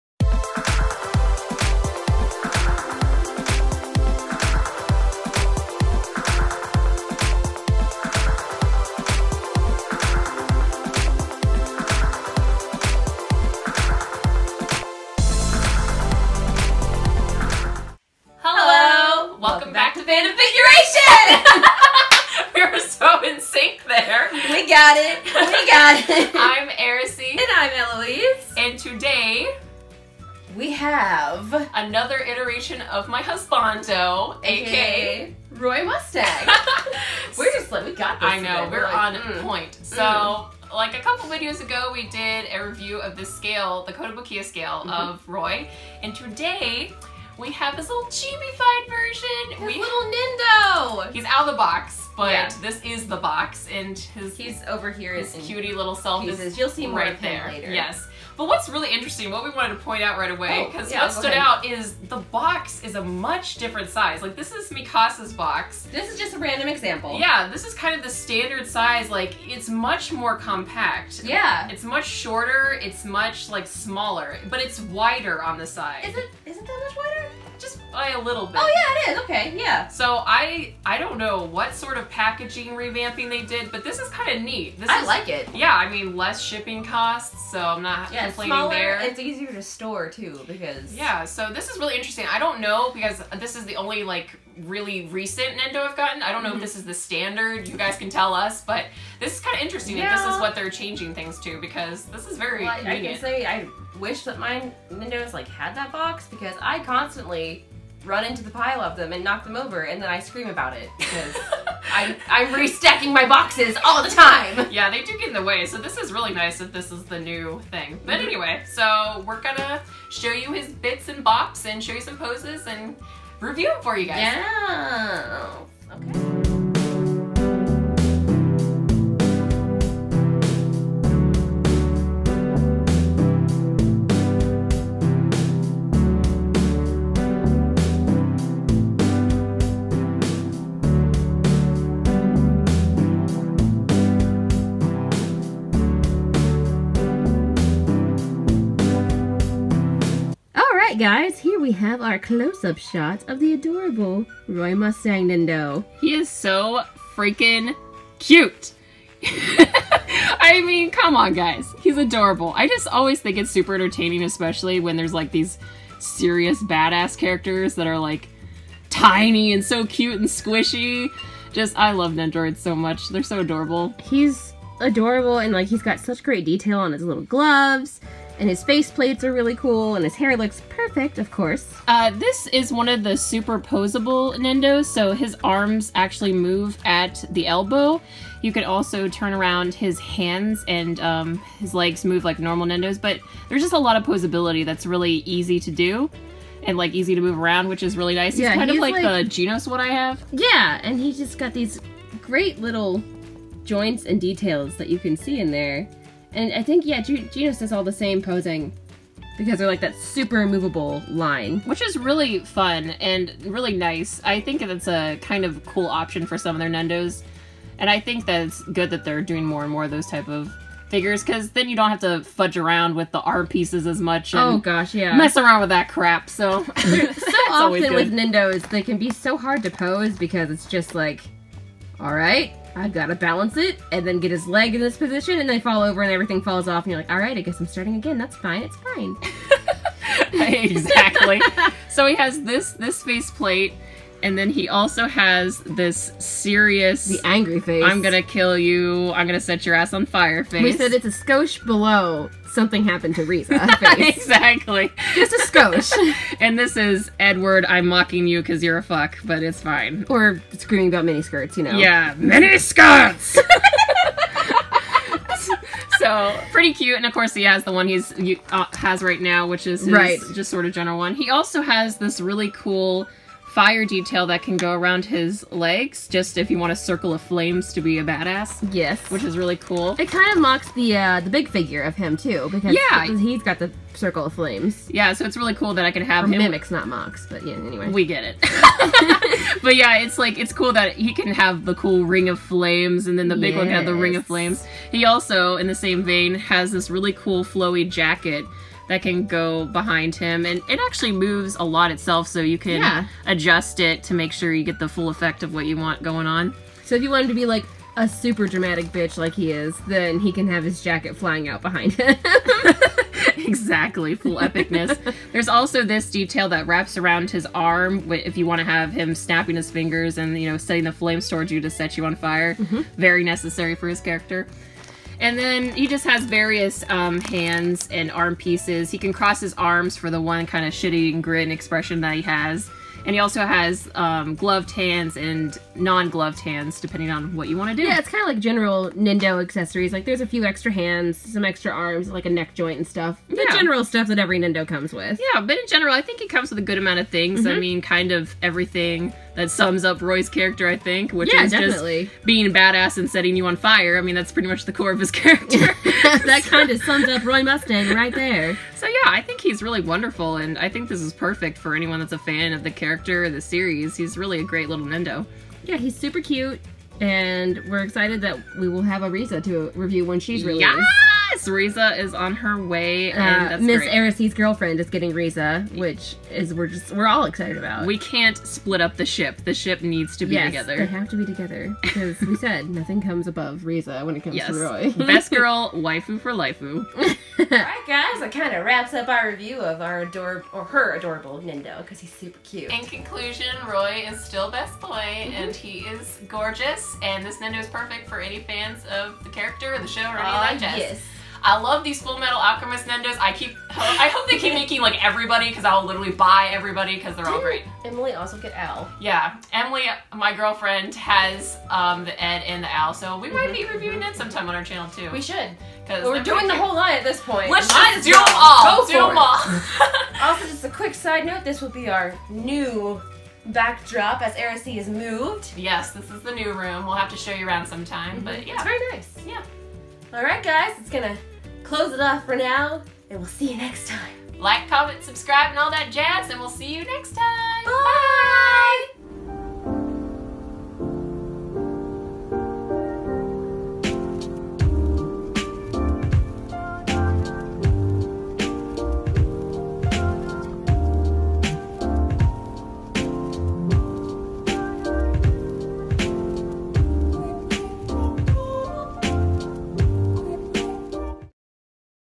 Roy Mustang Nendoroid Review
Hey everybody! Back at it again with that Fullmetal Alchemist love. Today we've got the Flame Alchemist yet again---this time as a chibi little nendo!